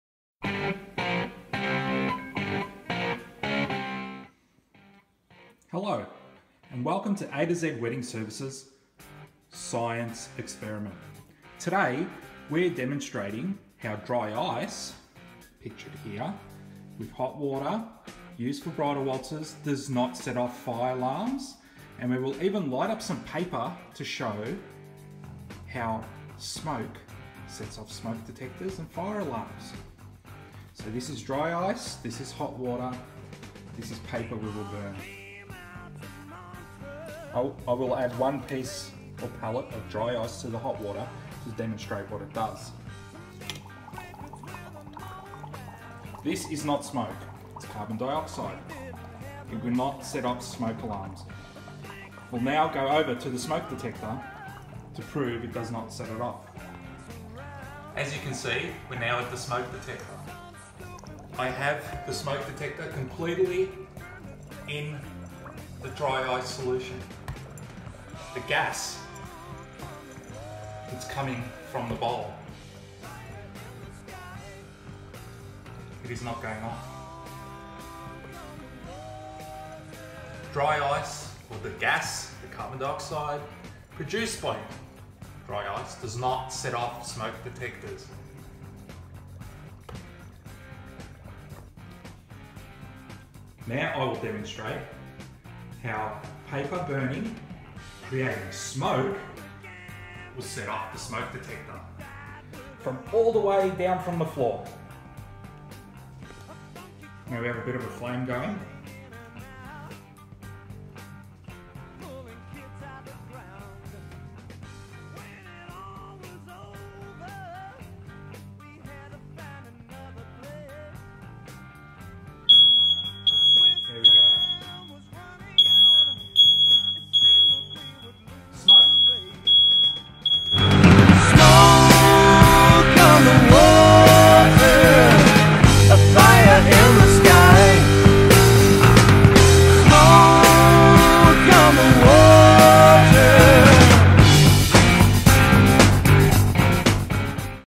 The Difference Between Dry Ice & Smoke
This Video explains that Dry Ice does not set off smoke detectors at wedding venues when it is used as a feature for the first dance.